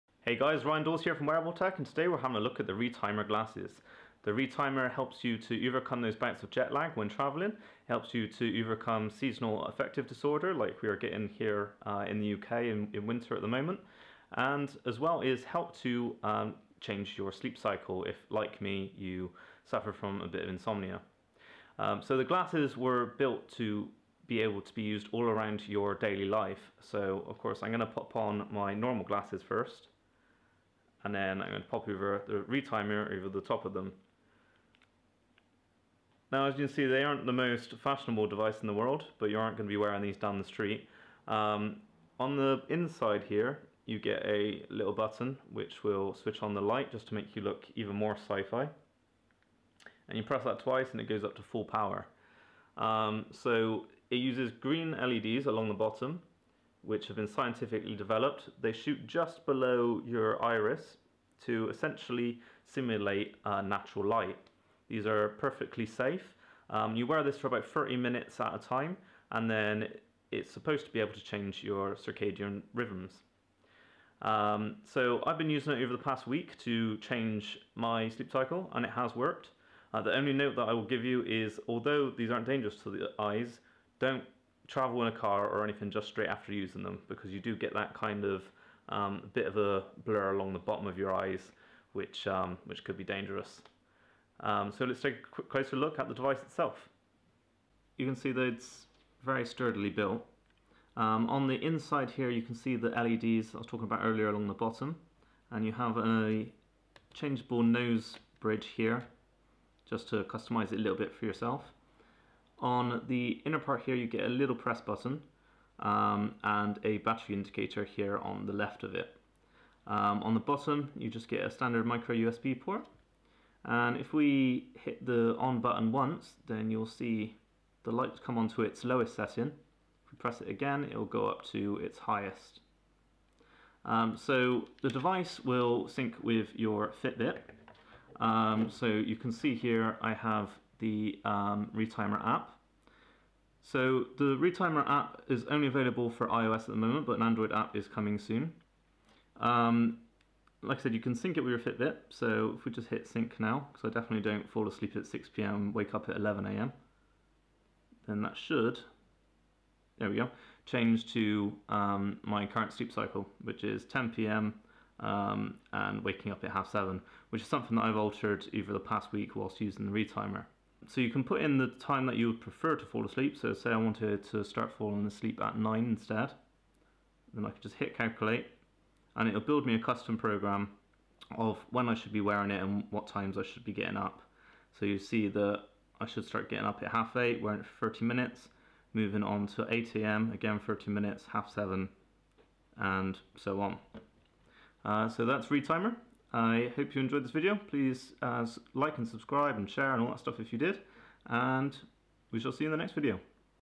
ReTimer Sleep Wearable (w/ Fitbit App) | TechForge Review
ReTimer is a new wearable device which aims to alter your sleep cycle through light therapy; perfect for helping with jet lag, insomnia, and SAD (Seasonal Affective Disorder.)

In this video, Ryan (@Gadget_Ry) goes hands-on with the device and takes a look at the accompanying app which syncs with Fitbit to change your current sleep habits.